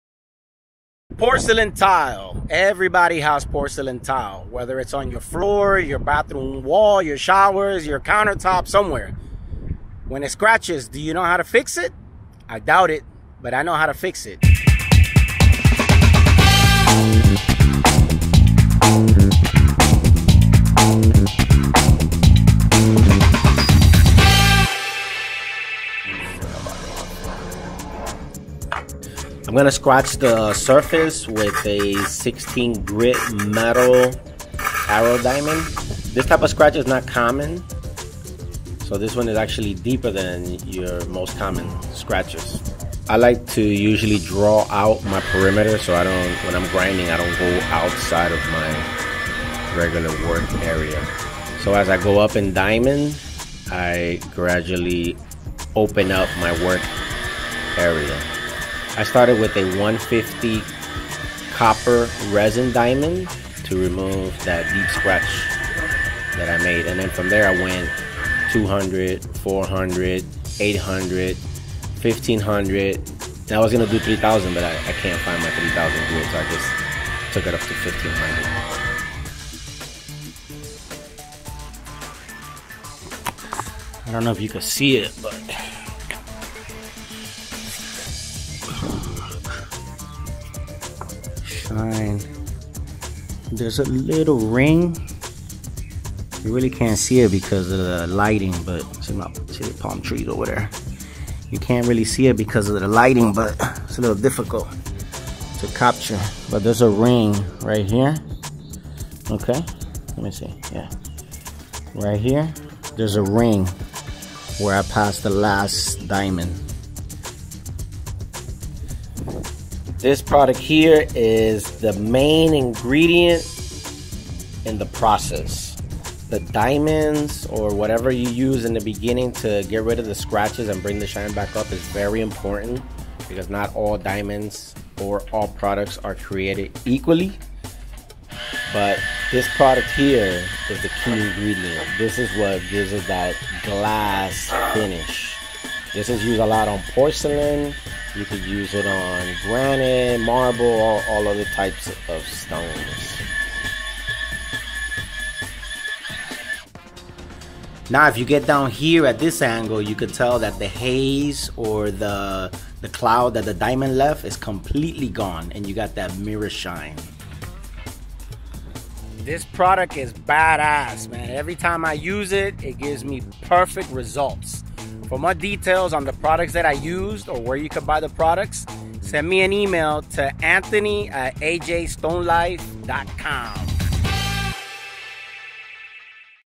How to polish Porcelain Tiles.
Learn the correct method to polish and restore Porcelain tiles. How to remove scratches from porcelain tiles.